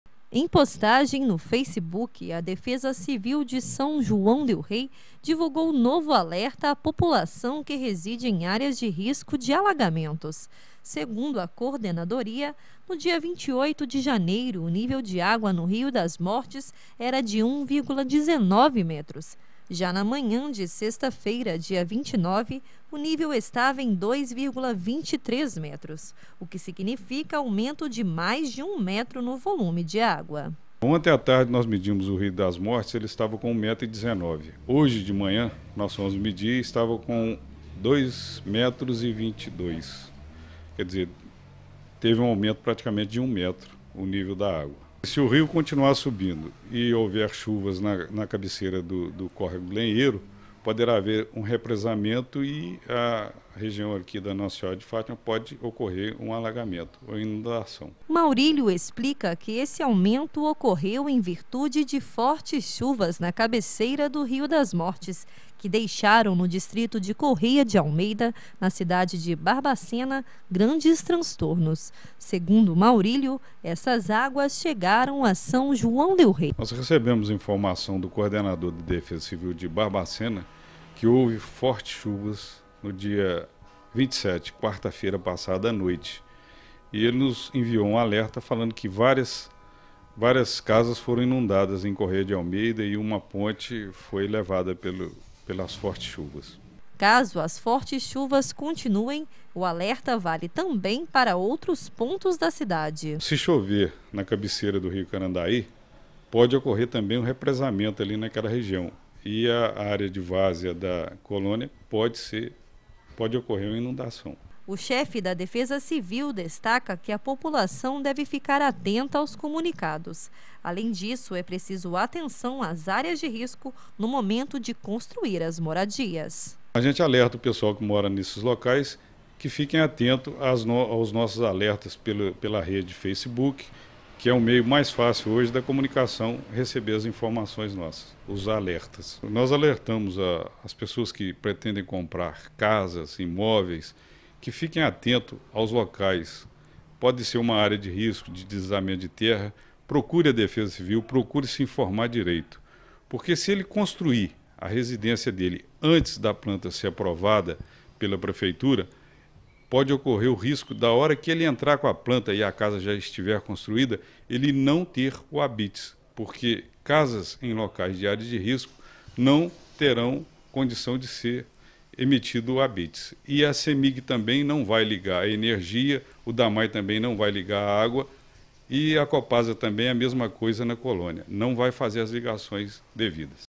Defesa Civíl alerta para o risco de aumento no nível do Rio das Mortes em SJDR 29/01/2016
Devido as fortes chuvas que tem atingido a região nos últimos dias, o nível de água no Rio das Mortes aumentou. a Defesa Civil de São João del-Rei está alertando a população sobre os riscos de alagamento em alguns pontos do município.